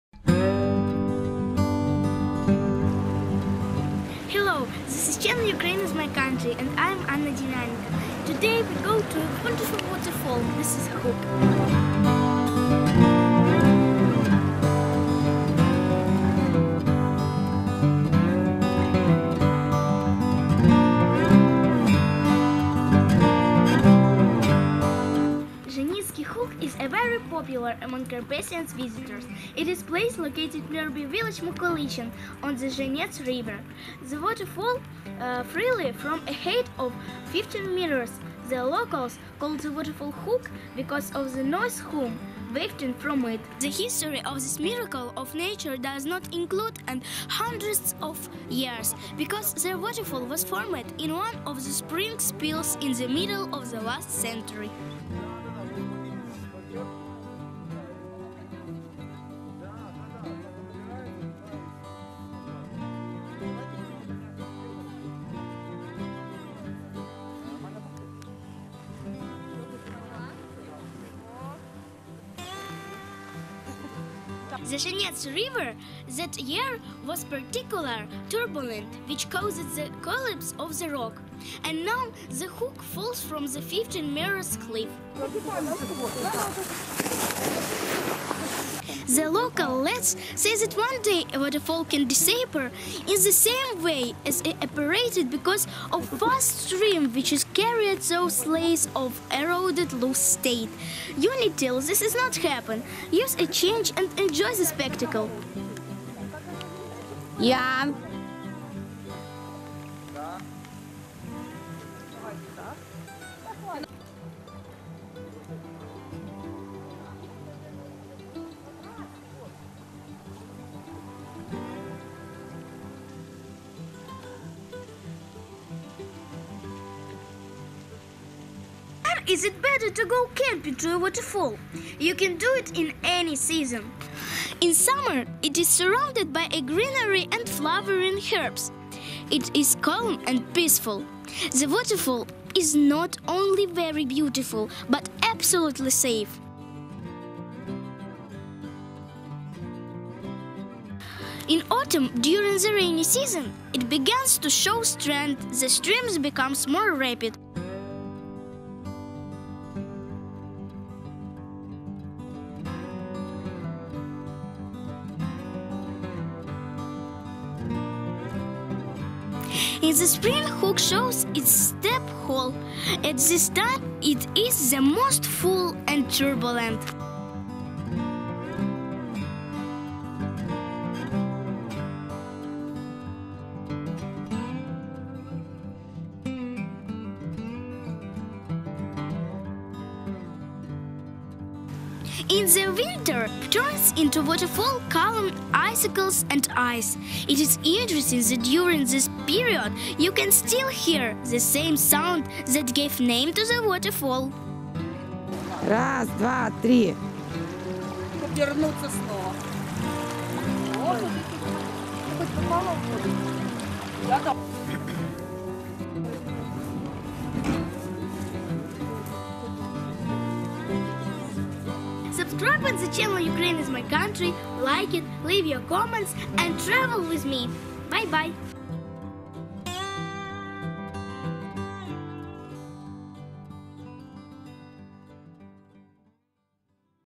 Journey to the Huk waterfall
Zhenetskyi Huk is very popular among Carpathians visitors.
It is place located nearby village Mykulychyn on the
Zhenet's river.The water falls freely from a height of 15
meters, the locals called the waterfall Hooke because of
the noise, hum, wafting from it.
The history of this miracle of nature does not include
and hundreds of years, because there waterfall was formed
in one of the spring spills in the middle of the last century.
The Zhenet's River that year was particularly turbulent,
which caused the collapse of the rock and now the Huk falls
from the  fifteen-meter cliff.
The local lads say that one day a waterfall can disappear
in the same way as it appeared because of fast stream which
is carried through layers of eroded loose slate. Until this
is not happen, use a chance and enjoy the spectacle.
When is it better to go camping to a waterfall?
You can do it in any season.In summer it is surrounded by
greenery and flowering herbs. It is calm and peaceful.
The waterfall is not only very beautiful, but absolutely safe.
 In autumn, during the rainy season, it begins to show strength,
the stream becomes more rapid and in the winter turns into a
waterfall column icicles and ice.
It is interesting that during this period you can still hear
the same sound that gave name
to the waterfall.In the spring Huk shows its steep hole,at
this time it is the most full and turbulent.